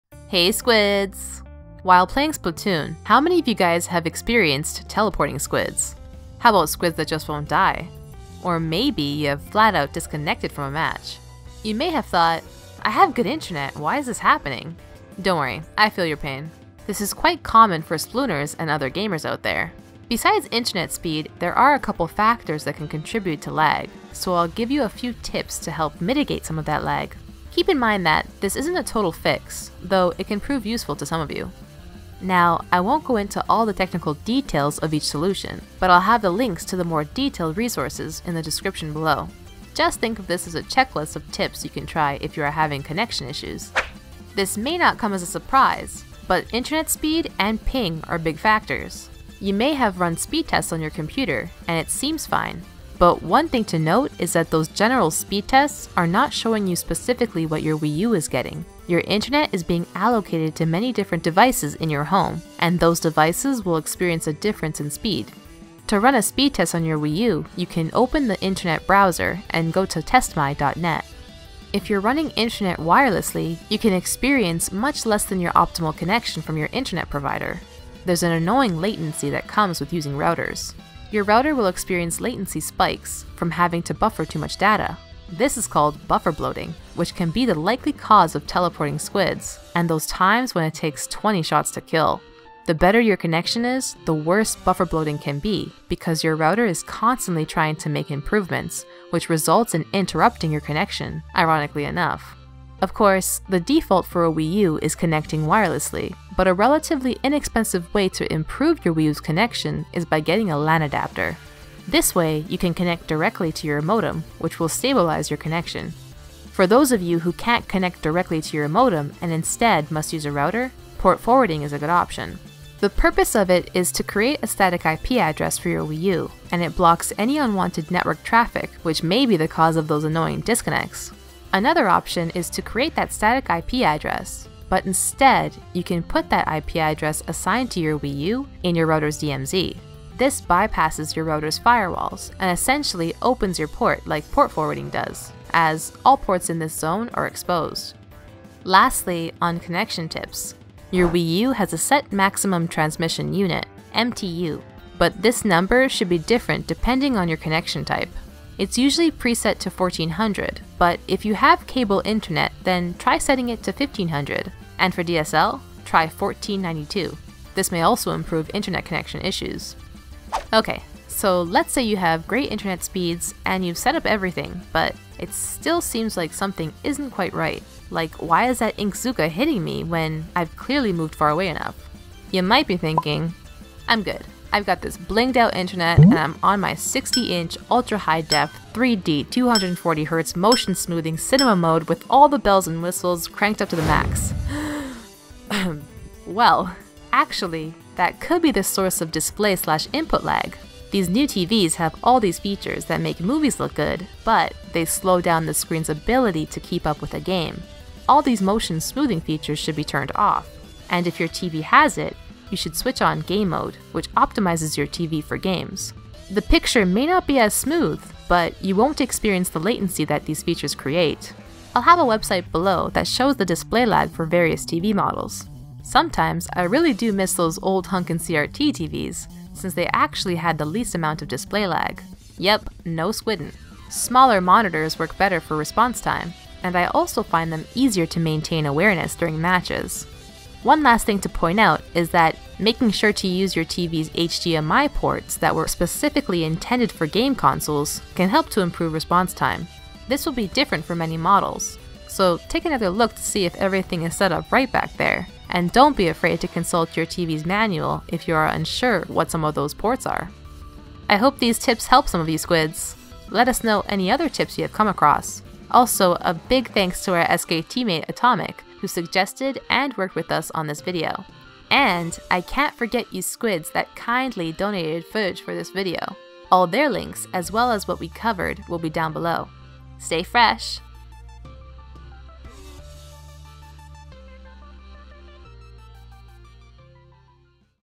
Squid Tip: Optimize Your Internet Connection!
Hey squids!  This time we are discussing ways to optimize your internet connection.  Let's help those laggy squids out there!